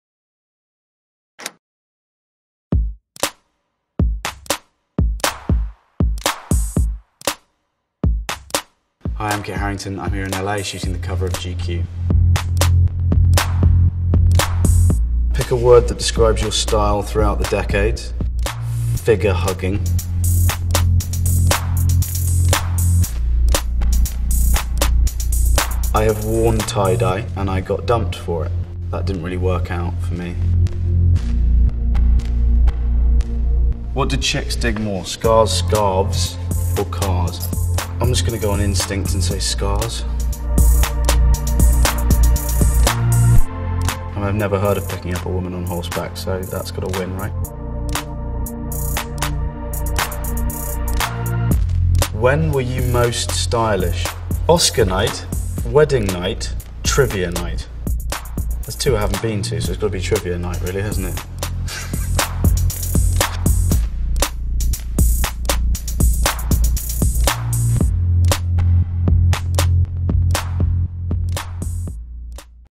Kit Harington The GQ Style Survey
DescriptionThe Game of Thrones star on his personal style, the one time he wore tie dye, and more